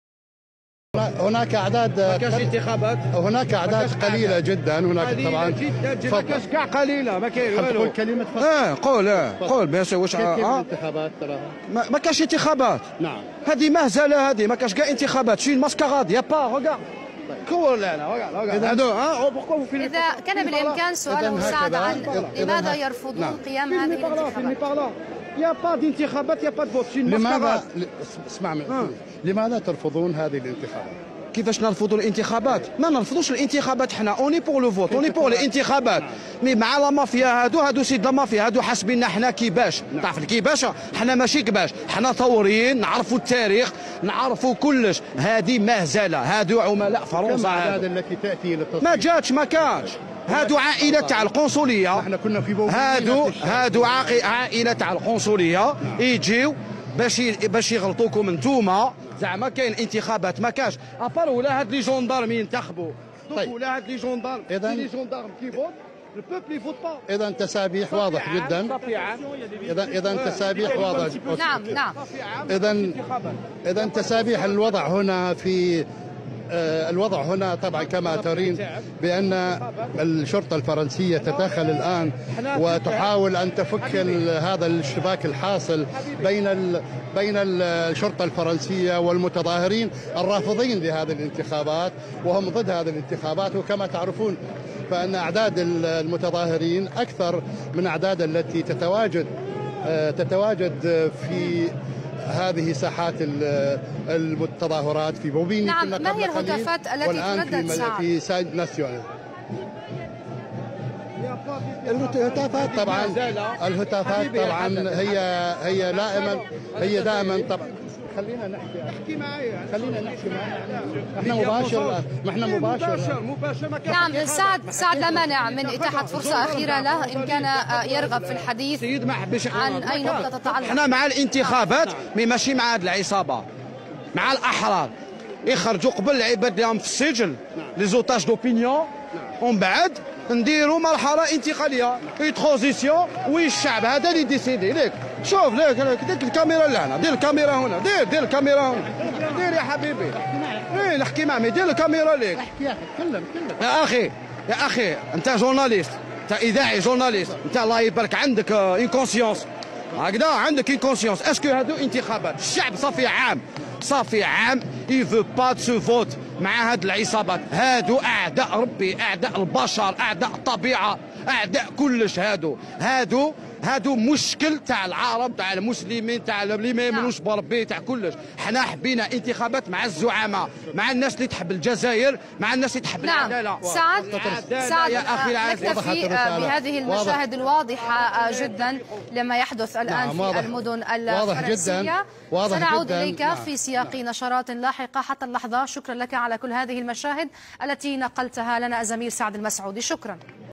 جزائري يتدخل و يقاطع صحفي قناة الحدث من أمام قنصلية جزائرية بفرنسا على طريقة "يتنحاو قاع
Ce brave jeune homme  interrompre la chaine... pas d'élections